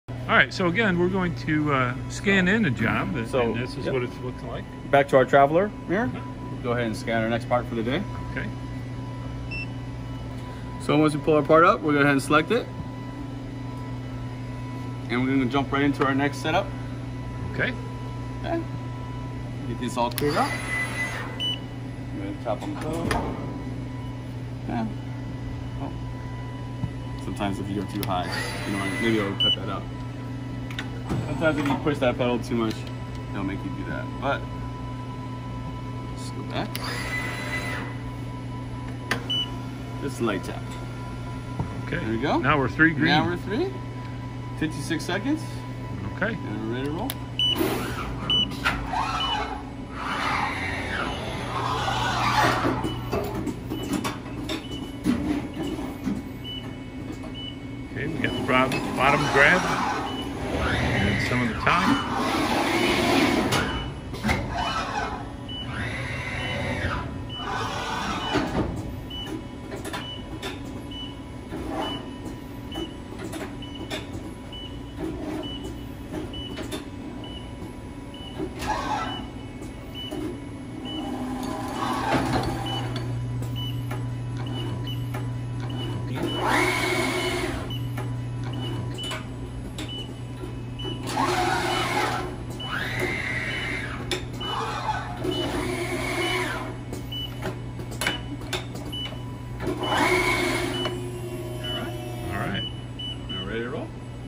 Amada HRB ATC #8 New job is scanned and set up 2
In this video we do our final setup, and get a good view of the ATC doing its job of serving up the tooling. We easily follow the Tool Layout Units which bring the tooling to its ultimate position for this job.